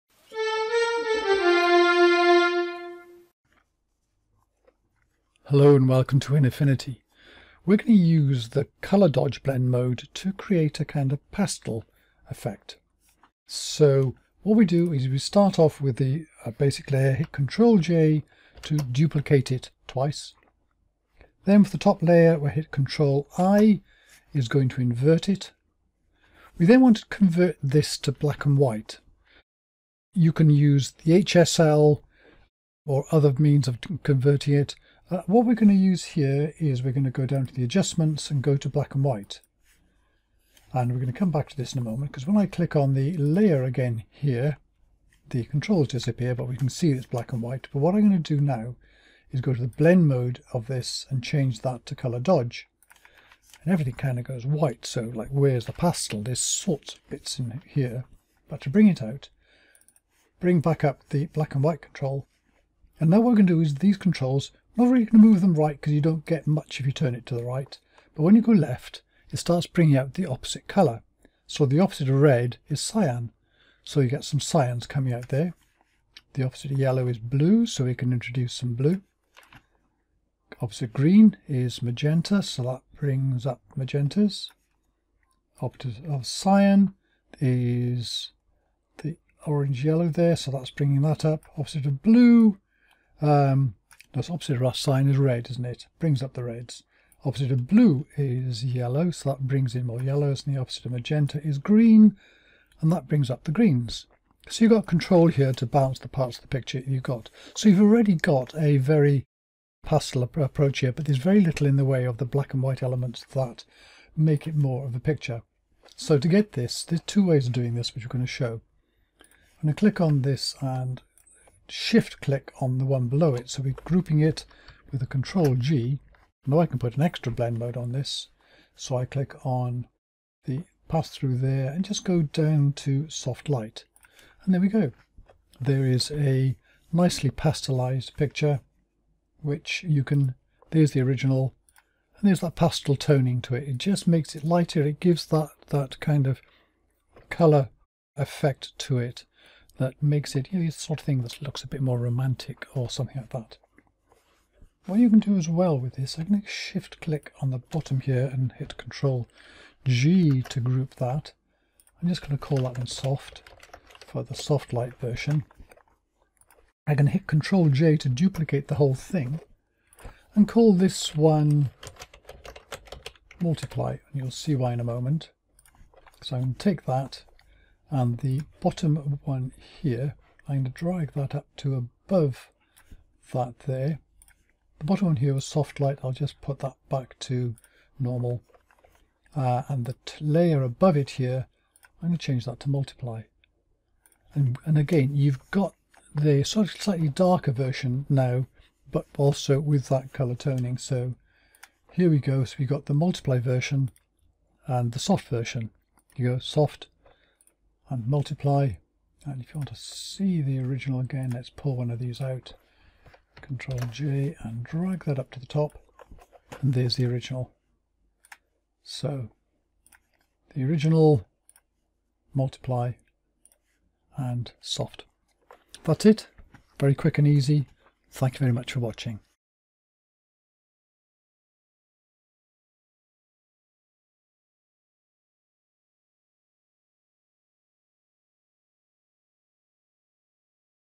Pastel Colour Grading Using Colour Dodge Blend Mode (using Affinity Photo)
Here's a way to create an adjustable pastel-graded picture using the Colour Dodge blend mode, plus variations on this using Soft Light and Multiply Blend Modes.

The web page for this video is here (with links to related videos)